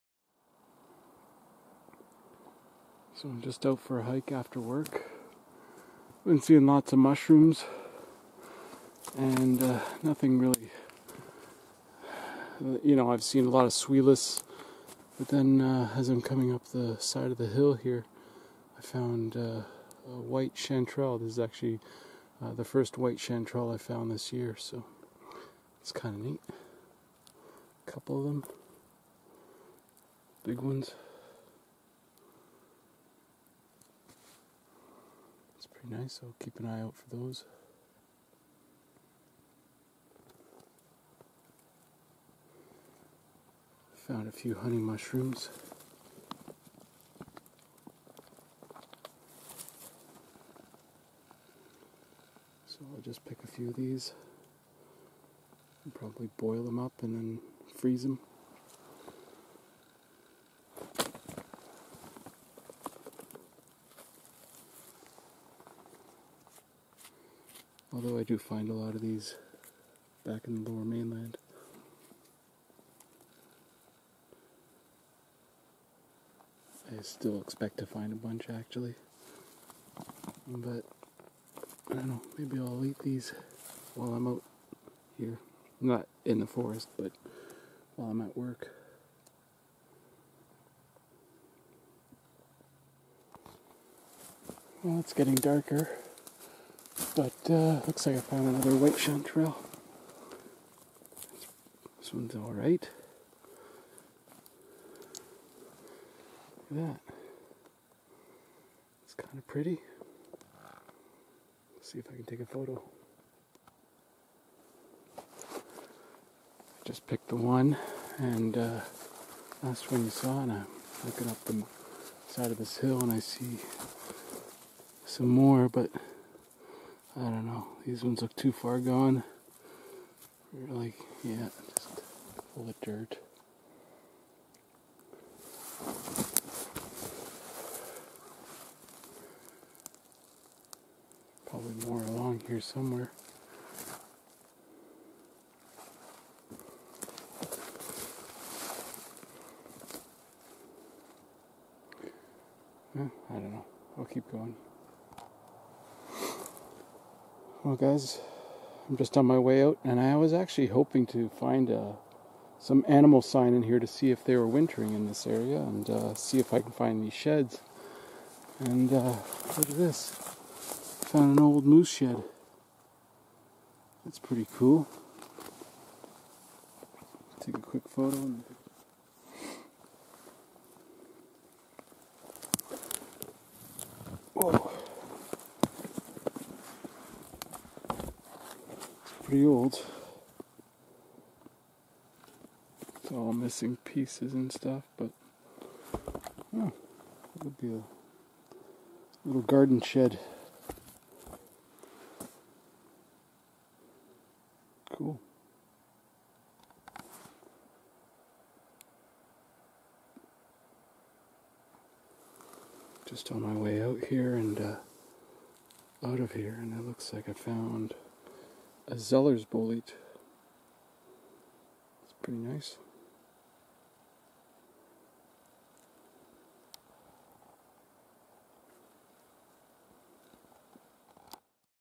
Foraging for Wild Mushrooms in Region 4 of BC 2021
Filmed on October 26, 2021 in British Columbia, Canada.